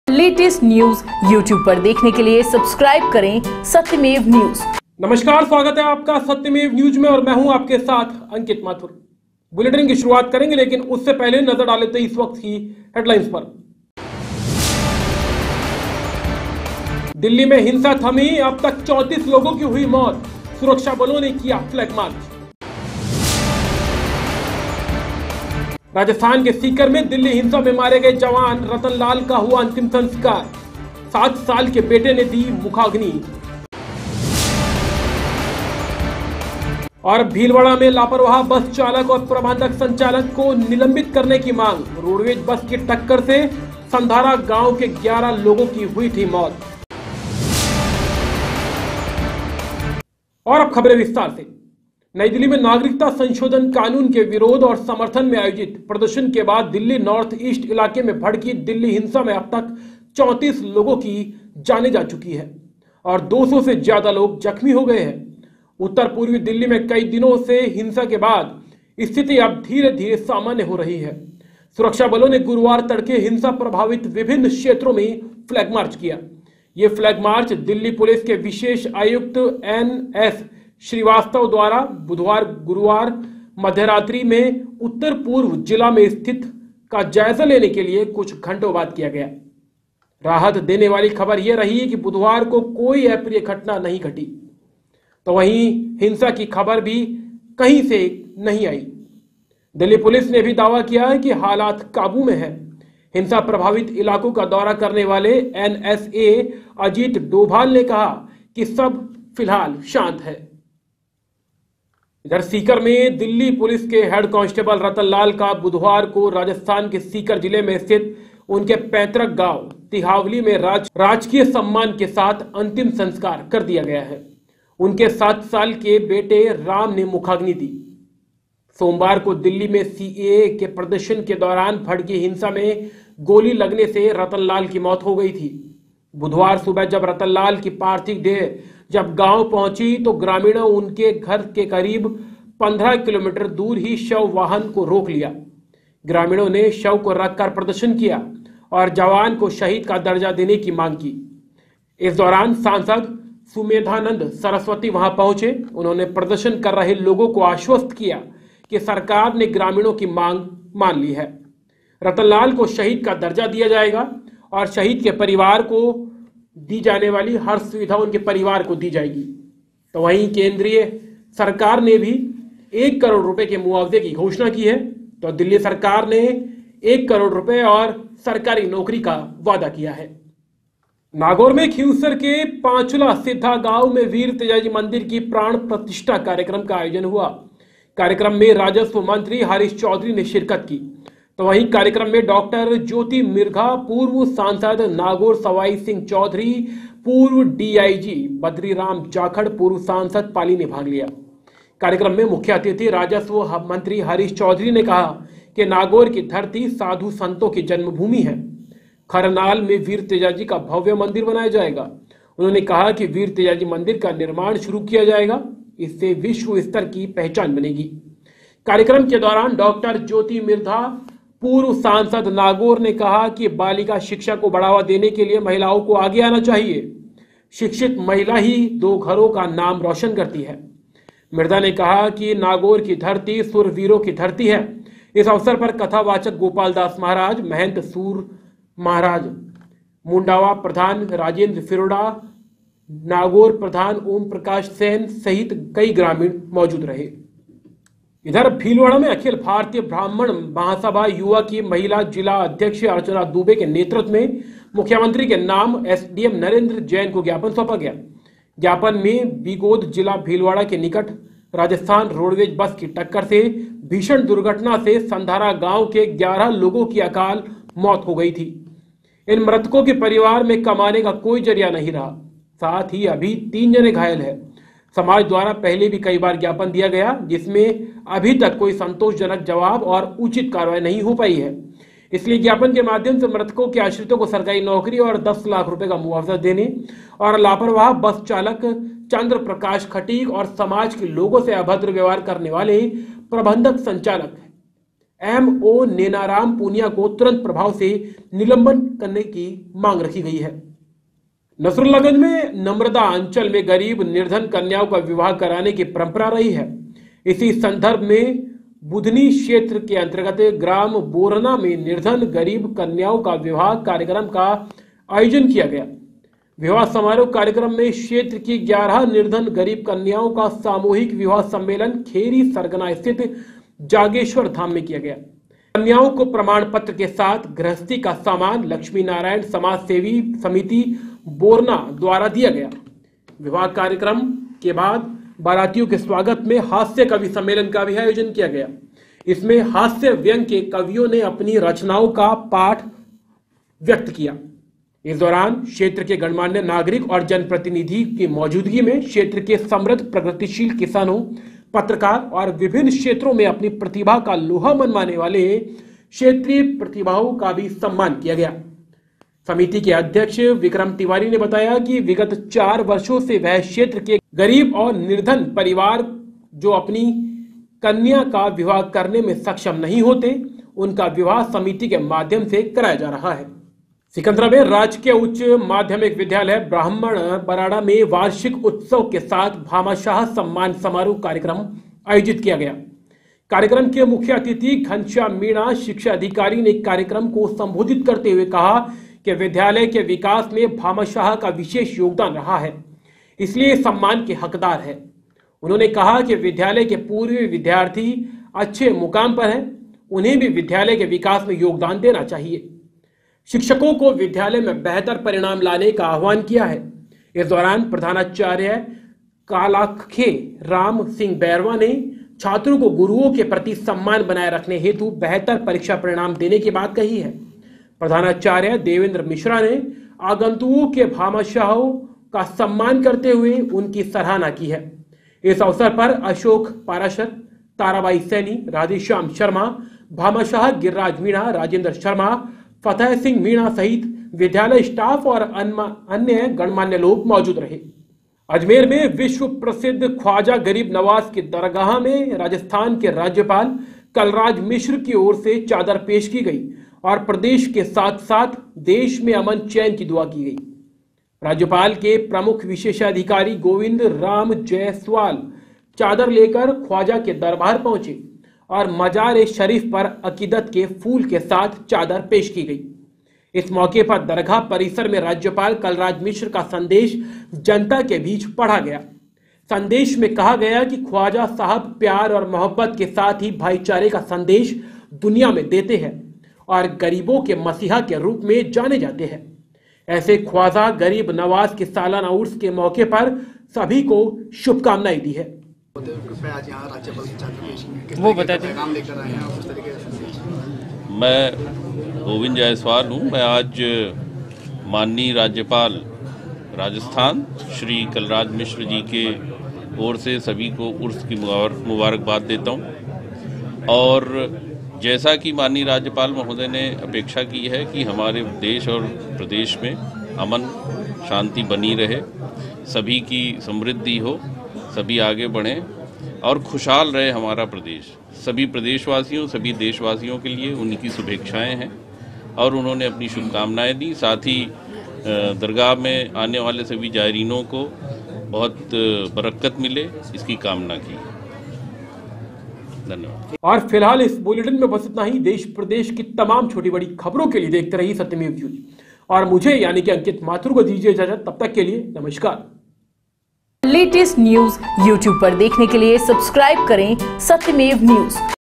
अब तक की सभी बड़ी ख़बरे 27 feb 2020
1 . नई दिल्ली
दिल्ली में हिंसा थमी, अब तक 34 लोगों की मौत
सुरक्षा बलों ने किया फ्लैग मार्च

2 . सीकर
दिल्ली हिंसा में मारे गए जवान रतनलाल का अंतिम संस्कार
सात साल के बेटे ने दी मुखाग्नि , मिलेगा शहीद का दर्जा

3 . नागौर

वीर तेजाजी मंदिर की प्राण प्रतिष्ठा कार्यक्रम का हुआ आयोजन

मंदिर निर्माण से विश्व स्टार पर बनेगी पहचान

4 . भीलवाड़ा

लापरवाह बस चालक और प्रबंधक संचालक को निलंबित करने की मांग

रोड़वेज बस की टक्कर से संधारा गांव के 11 लोगों की हो गयी थी मौत

5 . नसरुल्लागंज

11 कन्याओं का हुआ धर्म विवाह

बारातियों ने देर रात तक लिया कवि सम्मेलन का आनंद।

6 . सिकन्दरा

भामाशाह सम्मान समारोह का आयोजन

शिक्षकों द्वारा बेहतर परिणाम देने का आह्वान

7 . अजमेर

राज्यपाल ने अजमेर दरगाह में पेश की चादर

देश और राज्य के लिए मांगी अमन चैन की दुआ

गोविंदराम जयसवाल प्रमुख विशेष अधिकारी राज्यपाल